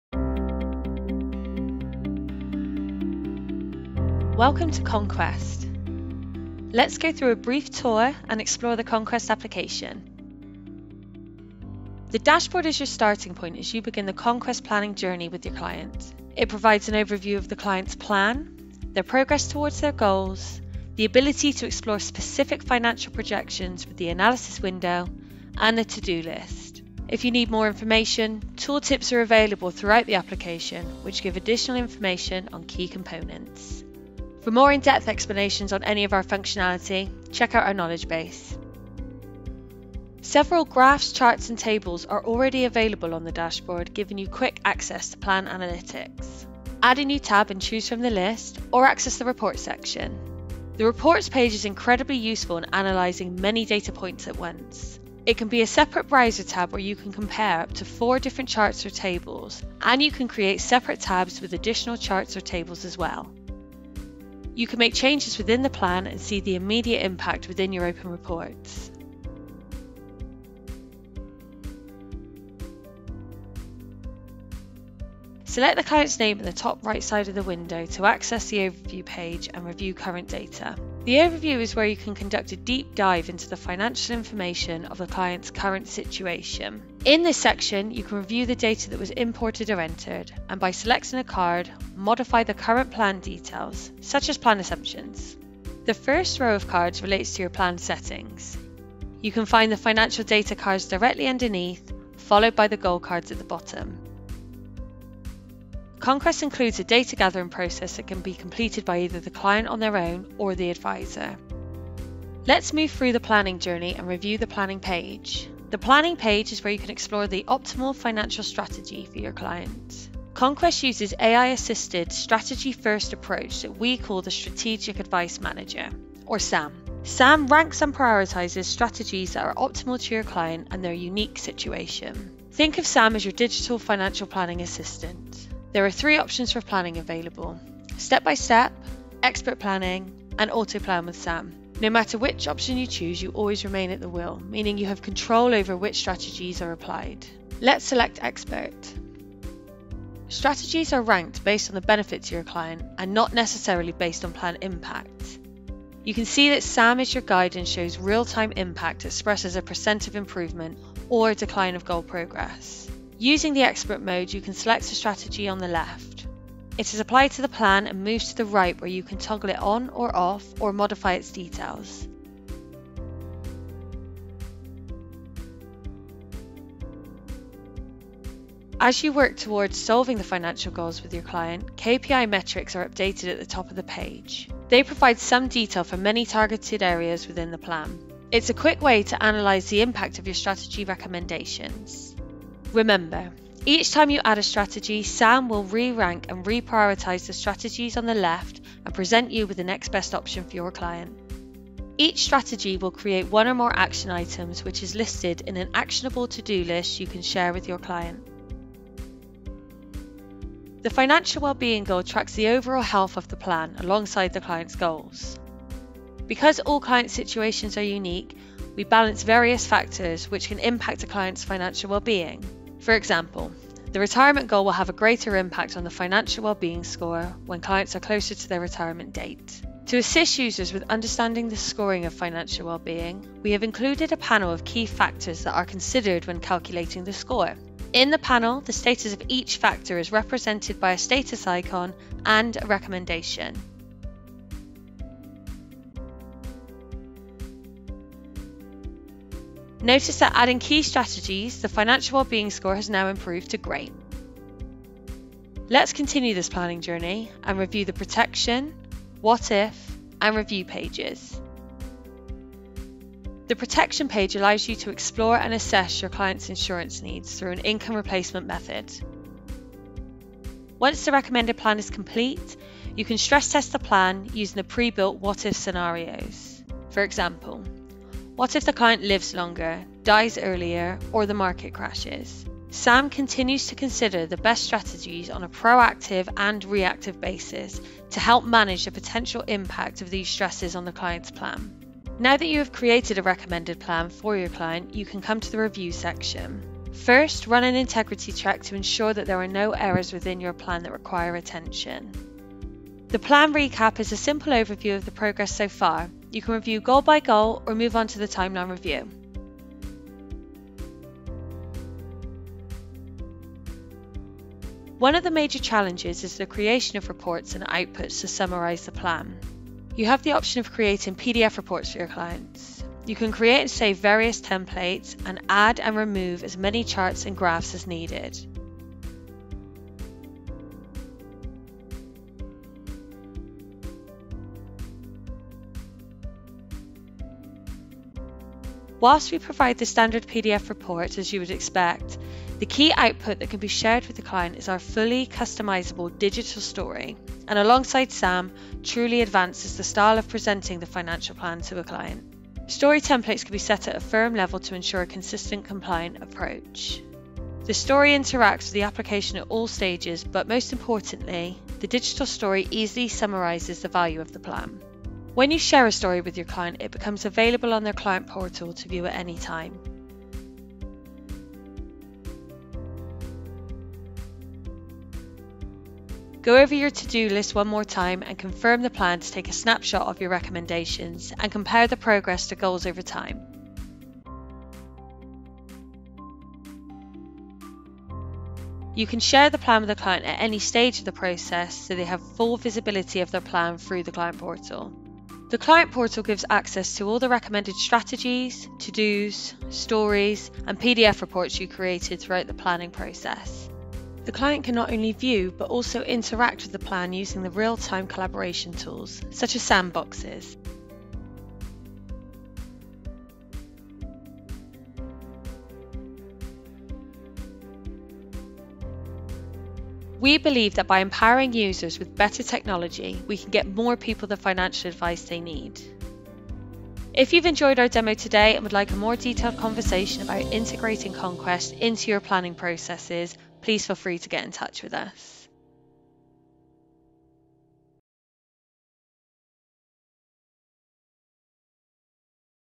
Conquest Tour - The Planning Journey (UK)
Take a peek inside our highly intuitive financial planning software. Discover how Conquest Planning can help accelerate the time it takes for you to deliver complex, hyper-customized financial advice. Spend less time planning and deliver better financial outcomes by leveraging automated financial planning strategies based on complex calculations and AI. Enjoy better client relationships when you invite clients to play with the plan in their very own sandbox account.

Let Conquest Planning do the heavy lifting so you can spend more time doing the things you love.